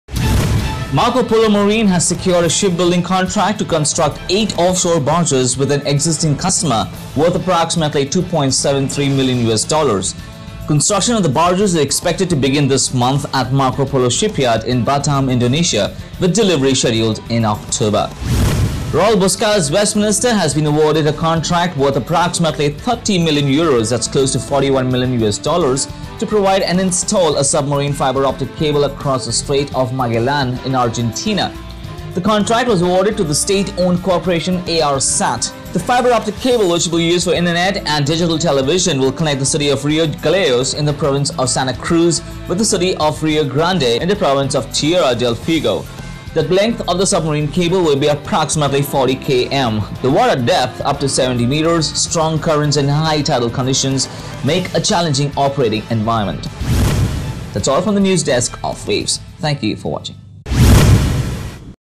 Marco Polo lands contract for eight offshore barges
Marco Polo Marine has secured a ship-building contract to construct eight offshore barges with an existing customer, worth approximately US$2.73 million. Construction of the barges is expected to begin this month at Marco Polo's shipyard in Batam, Indonesia, with delivery scheduled in October.  Royal Boskalis Westminster has been awarded a contract worth approximately €30 million (US$41 million) to provide and install a submarine fibre-optic cable across the Strait of Magellan, Argentina.